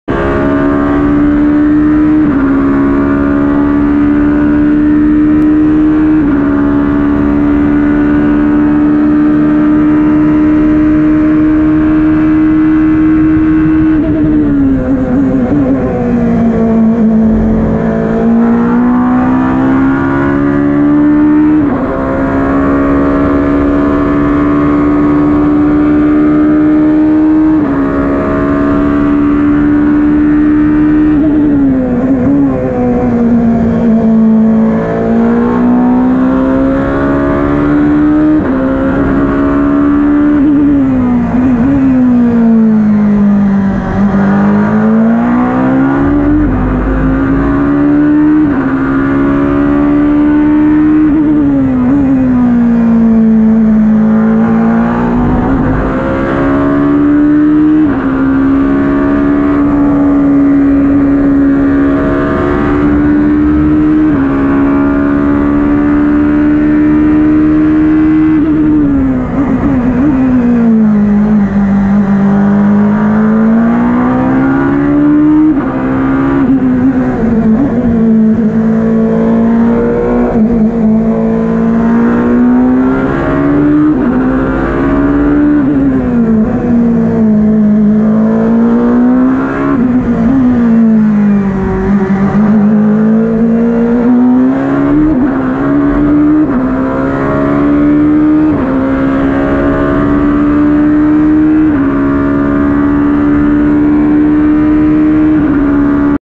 Ligier JSP2 LMP2 Barcelona POV Onboard
Onboard an ex Chris Hoy Ligier JSP2 Judd at Barcelona :)